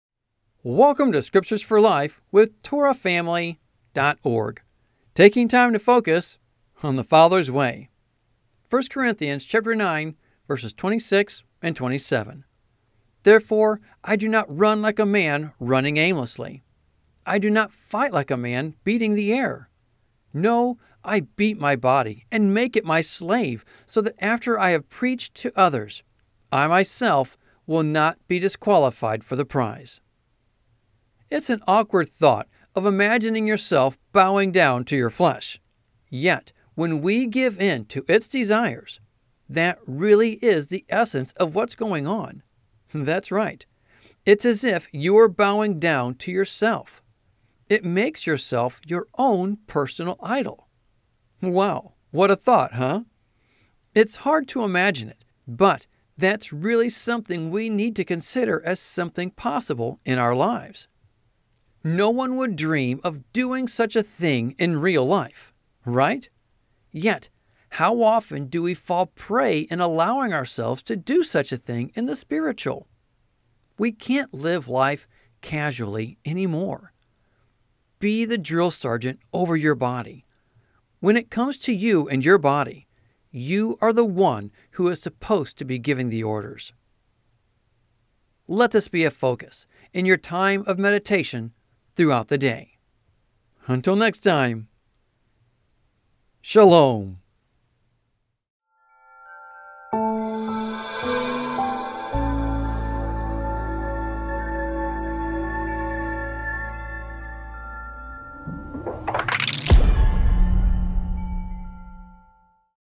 Scriptures for Life | Giving Orders
Daily devotion for May 14 (2 min)

1 Corinthians 9:26 Therefore I do not run like a man running aimlessly; I do not fight like a man beating the air.  27 No, I beat my body and make it my slave so that after I have preached to others, I myself will not be disqualified for the prize.